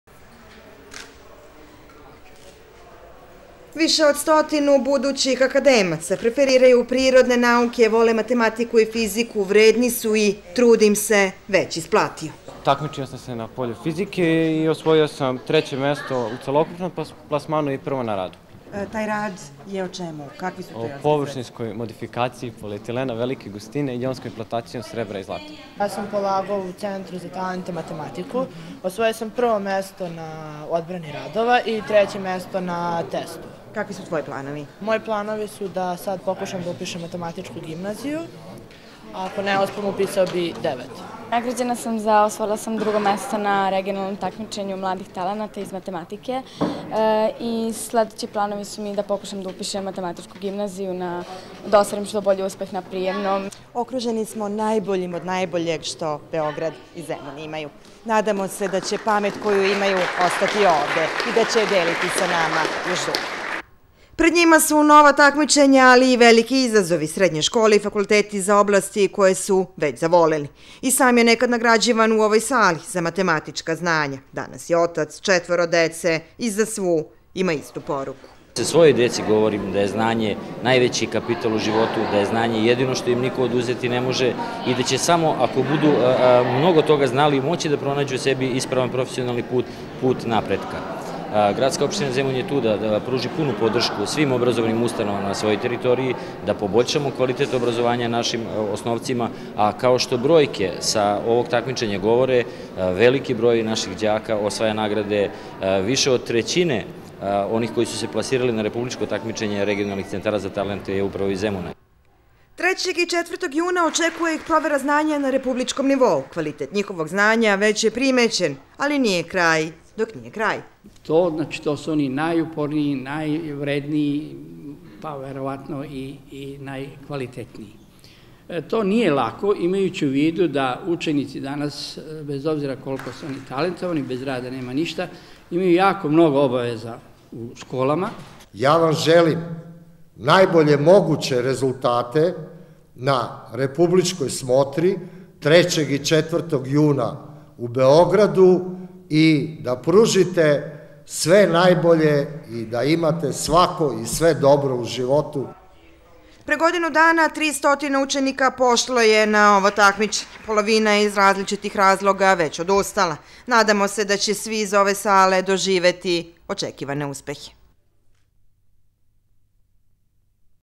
Nagrade u Zemunu najuspešnijima na takmičenju za regionalne talente
Učenicima osnovnih i srednjih škola koji su osvajali nagrade na takmičenju za regionalne talente uručene su nagrade u opštini Zemun. Znanje 156 istrajnih učenika se isplatilo, jer su kako kažu u opštini, velike mogućnosti pred njima.